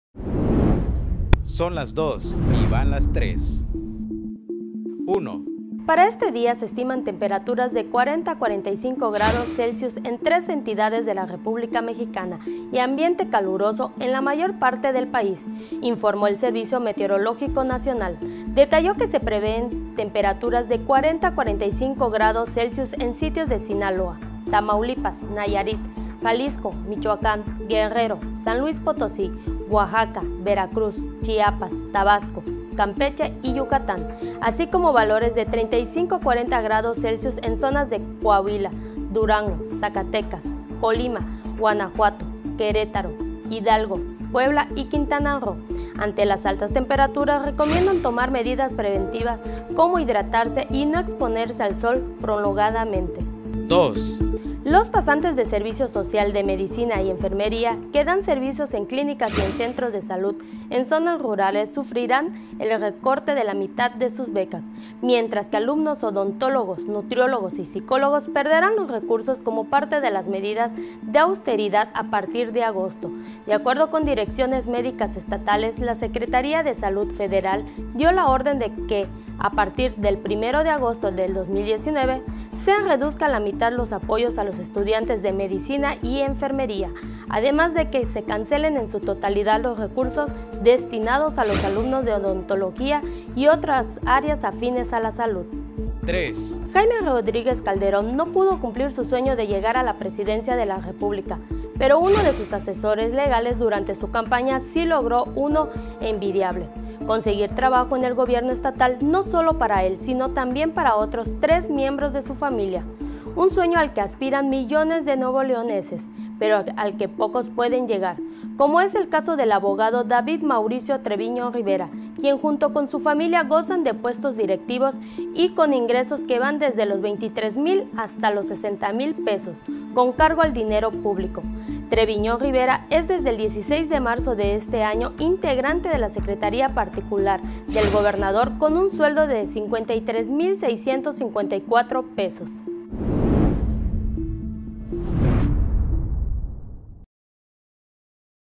ESPERAN 13 ENTIDADES CALOR DE HASTA 45 GRADOS
Son las dos y van las tres:
- POR AUSTERIDAD, REDUCIRÁN BECAS A PASANTES DE MEDICINA Y ENFERMERÍA
- PREMIA ‘EL BRONCO’ A ABOGADO Y A SU FAMILIA CON SUELDAZOS